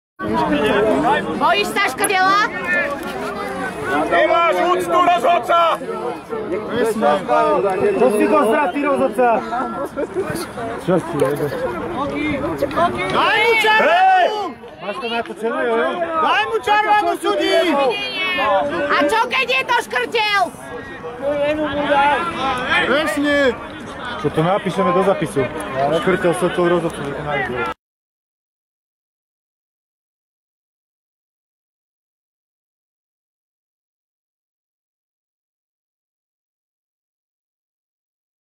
MUST SEE 🔴 Ex-Liverpool player SKRTEL pushed the referee! 😲
Former @LiverpoolFC player MARTIN ŠKRTEL pushed the referee, but was seen only yellow card 🟨 Was it fight?
Martin Škrtel spent eight-and-a-half years with Liverpool, where he played 320 official games and scored 18 goals from 2008 to 2016.
He retired from professional footbal in 2022 and now, he is playing for his boyhood amateur team Ráztočno in 8th Slovak league.

Škrtel joined FC Liverpool on 11 January 2008 on a four-and-a-half-year contract for a transfer fee of £6.5 million and was signed by then Liverpool manager Rafael Benítez.